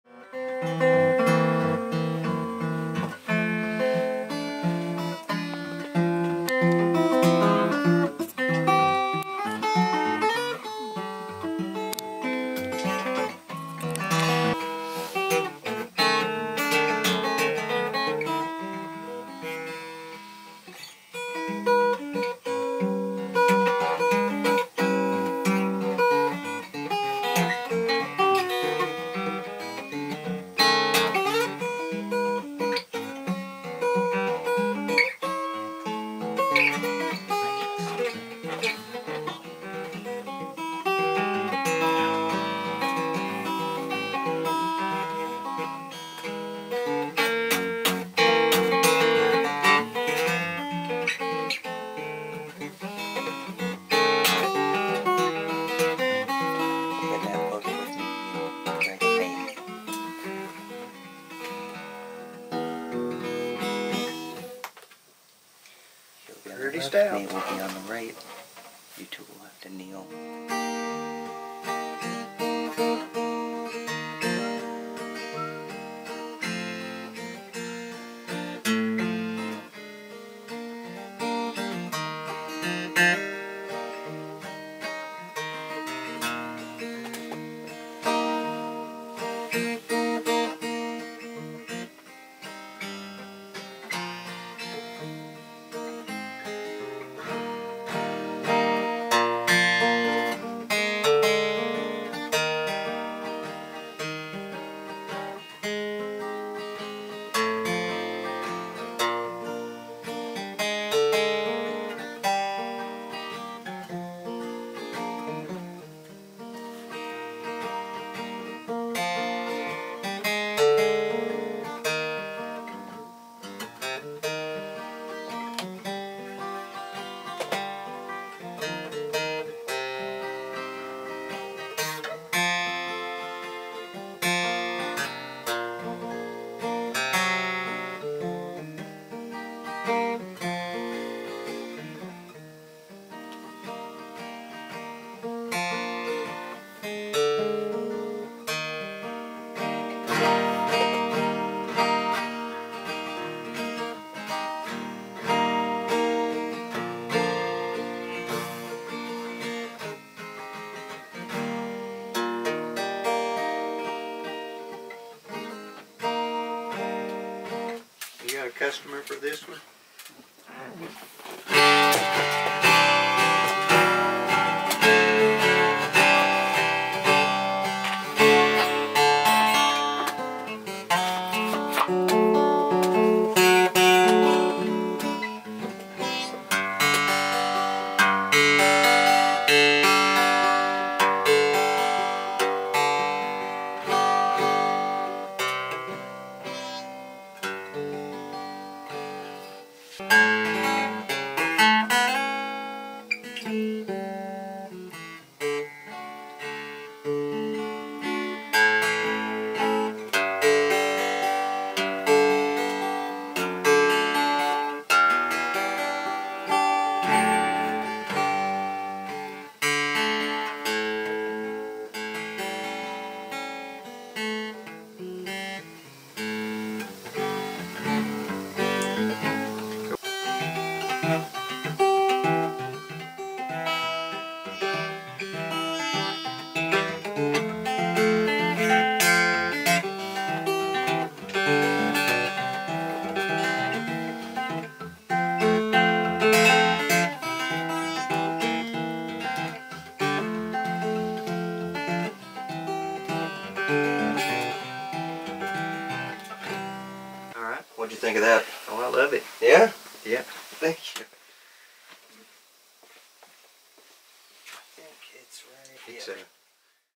John Arnold checks out Siren Acoustic Guitars
Greg and Josh of Siren Acoustic Guitars LLC took a Trip to TN this past August and visited with a few legends in the Luthier world. One of which was none other than John Arnold himself!

This video shows John Arnold playing a few of the Guitars Greg and Josh brought with them on their trip.